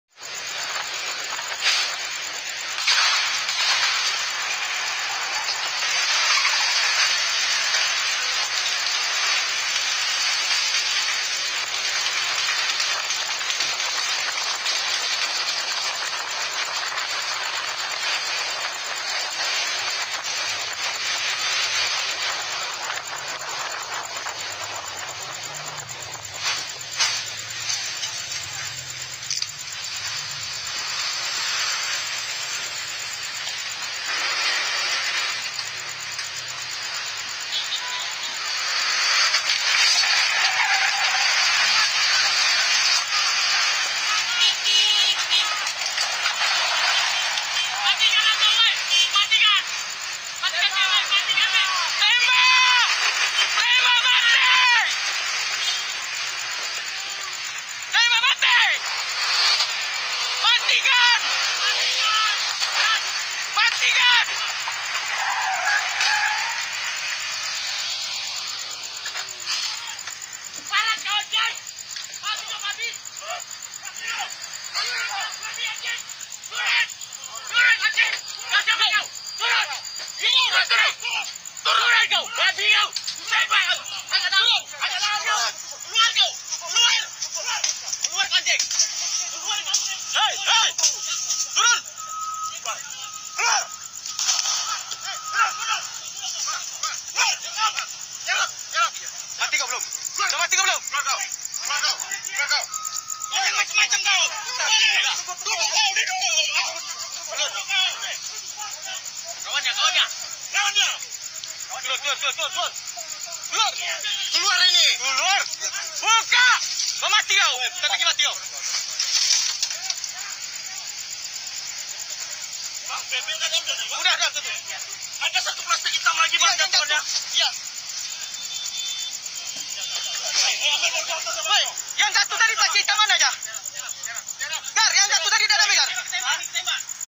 Detik Detik Penangkapan Kurir Narkoba Di Riau
aksi kejar kejaran kurirnarkoba yang melibatkan oknumpolisi di